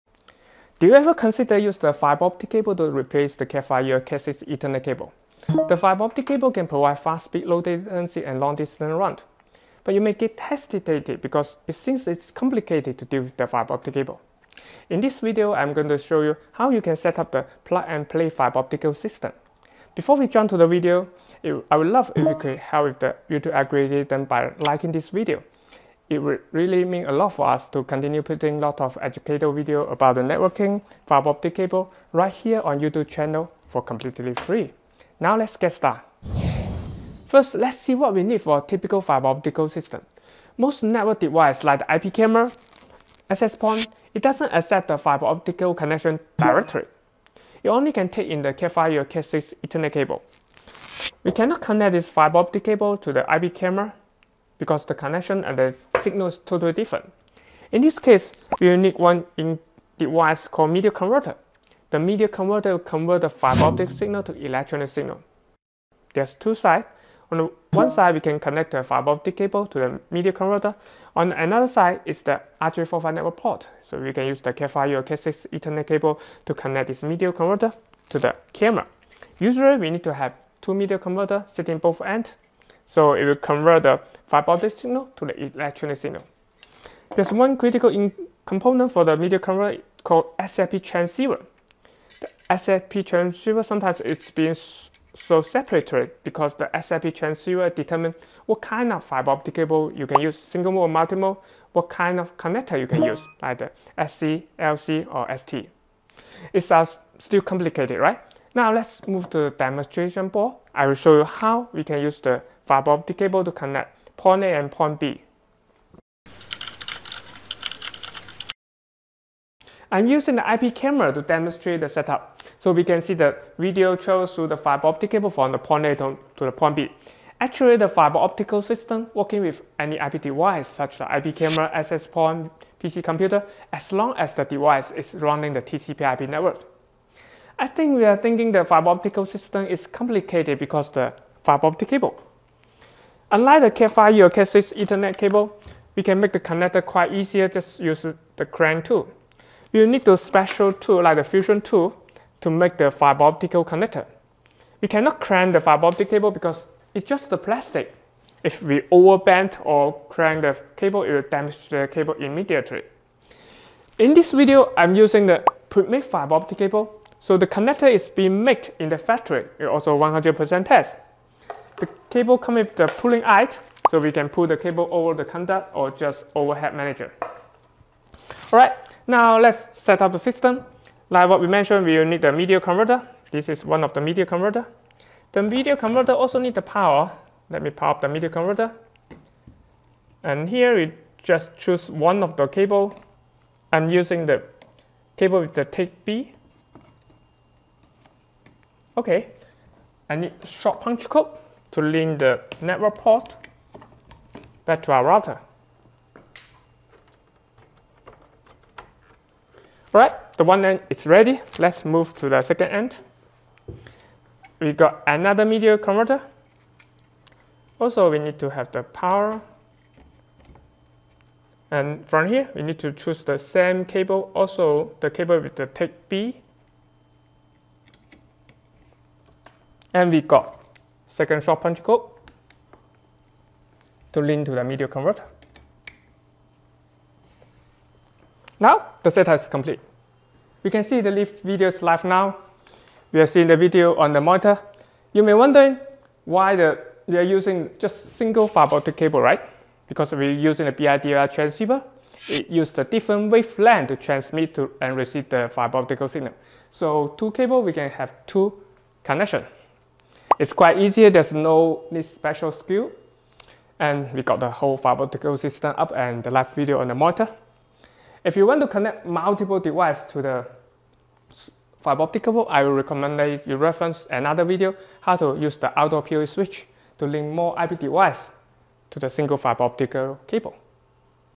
Fiber VS Cat6 Is it Fiber Optic System Setup Complicated？
Fiber optic network provides long distance, fast speed and low latency. Some people think fiber optic termination is complicated as you know few about fiber optic network system setup. Fiber or copper connection, which to choose is question. This video will walk you through the needed devices in a fiber optic network system and tell you how to build a plug-and-play fiber optic system from point A to point B.

0:00:00 Why Choose Fiber Optic Network?
0:00:36 What Devices Do You Need in Fiber Optic Network System Setup?
0:02:21 Why Fiber Termination is Complicated?
0:02:43 Pre-terminated Fiber Optic Cable Introduction
0:02:58 How to Build a Point-to-Point Fiber Optic System?
0:04:28 What to Do If You Need to Install Multiple IP Cameras at Outdoors?